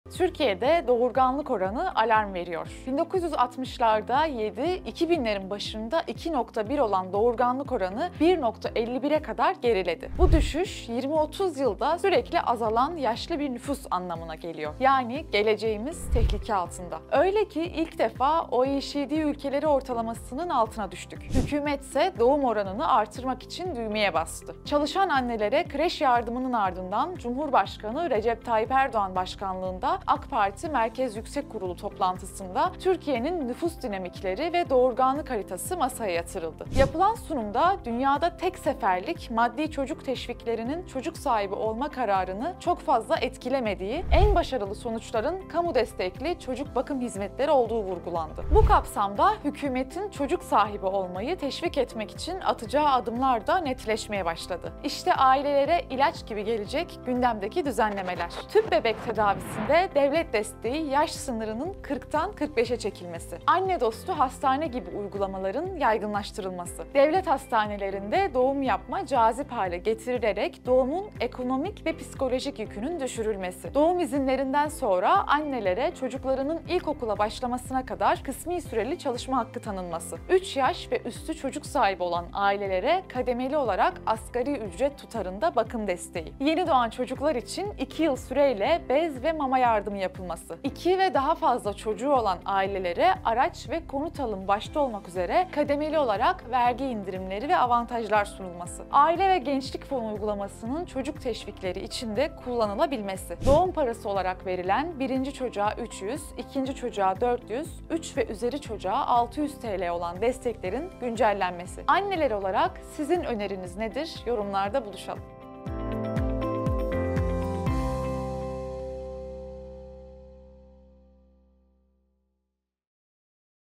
Ailelere Dev Yardım Yolda! Tek Şart: Çocuk! - Mavi Kadın
Türkiye İstatistik Kurumu’nun (TÜİK) verilerine göre, doğurganlık hızı, 1,51 ile nüfusun yenilenme düzeyi olan 2,1’in altında kaldı. Nüfus giderek yaşlanırken bu tablonun tersine çevrilmesi için bir takım çalışmalar başlatıldı. AK Parti MYK toplantısında da bununla ilgili dikkat çeken öneriler dile getirildi. Bunlardan biri ise 3 çocuklu ailelere 17 bin TL bakım desteği verilmesi. Diğer teşvik önerilerinin tamamını Mavi Kadın İçerik Editörü Dilara Tekin anlattı 🎙️